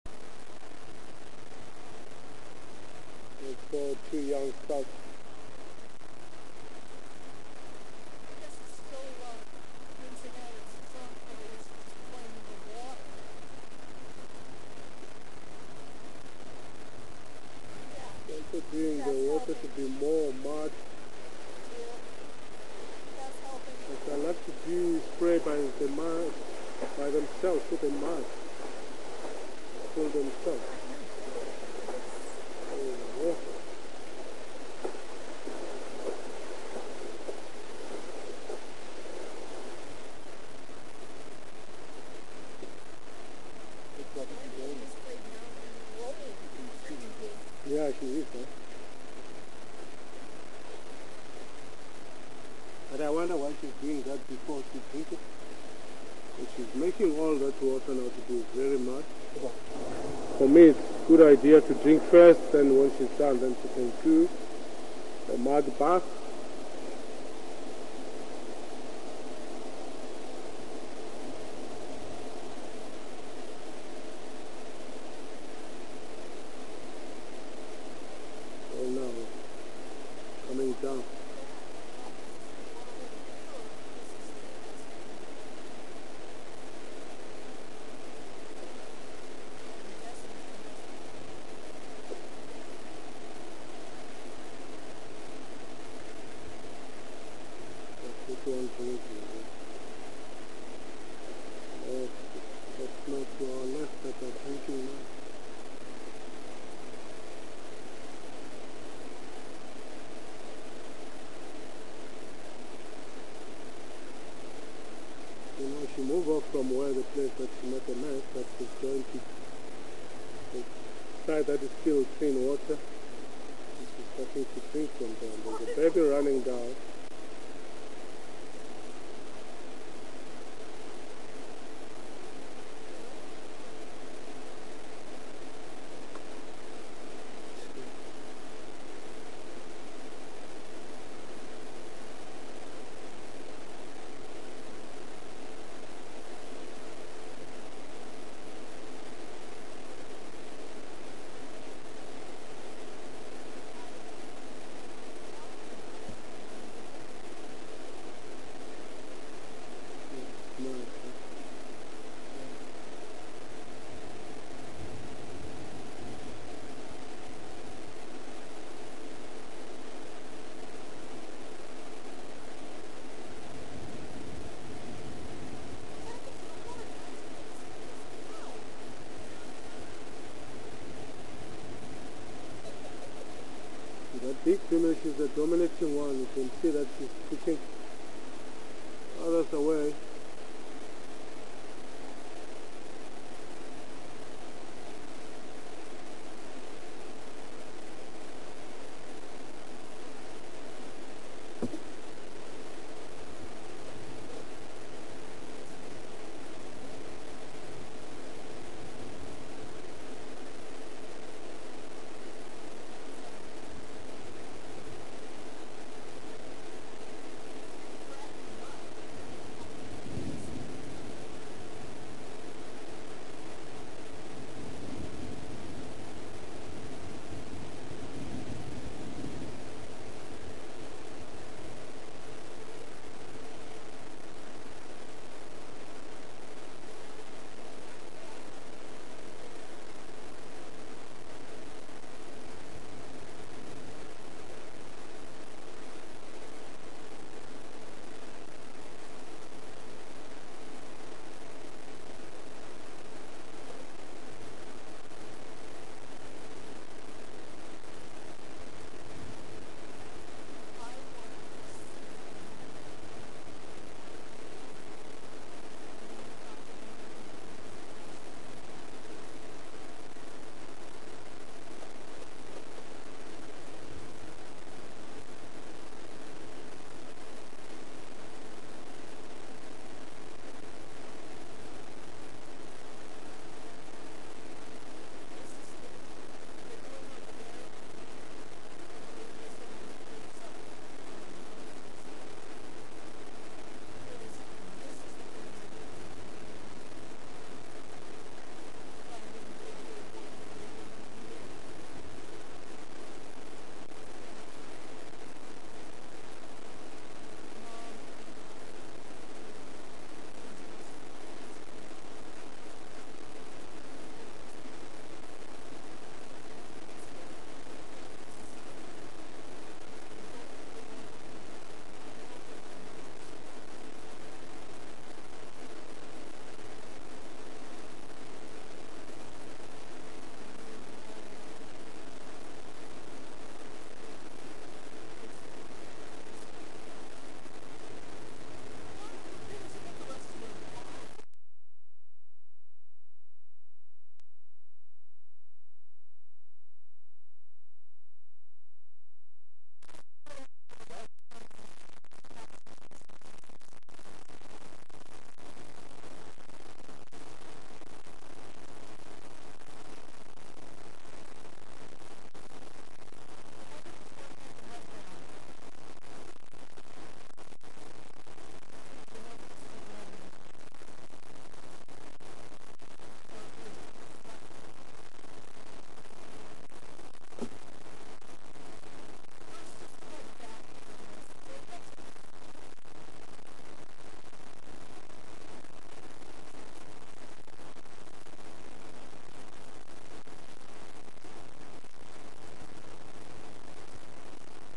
Elephant mudbath....couldn't you let us drink FIRST !!!!!
The first ellie to reach the water at Treehouse dam really mucked things up for the rest of this family group.